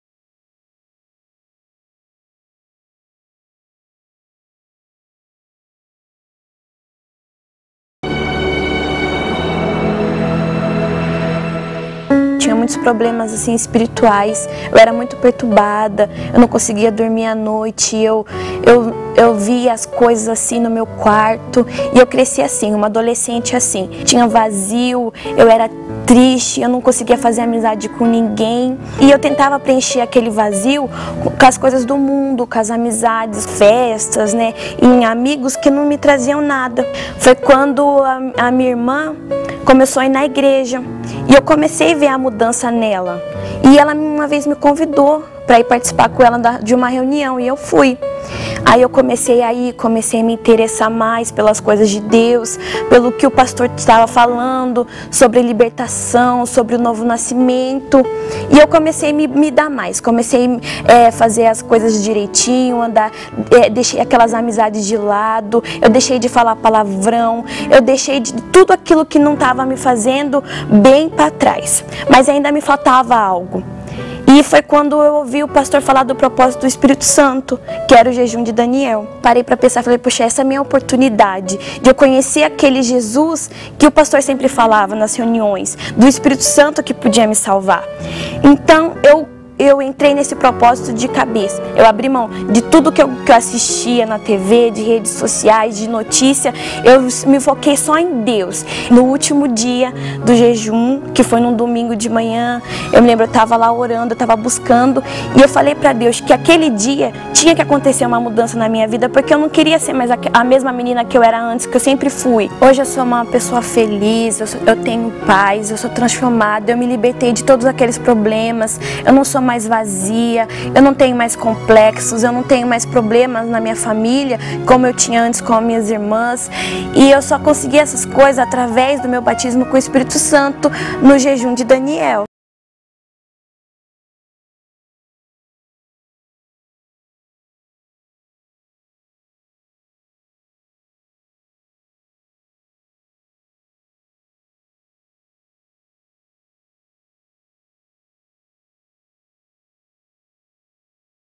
Eu tinha problemas espirituais | Jejum de Daniel
Durante o Jejum de Daniel ela conseguiu resolver todos os seus problemas espirituais.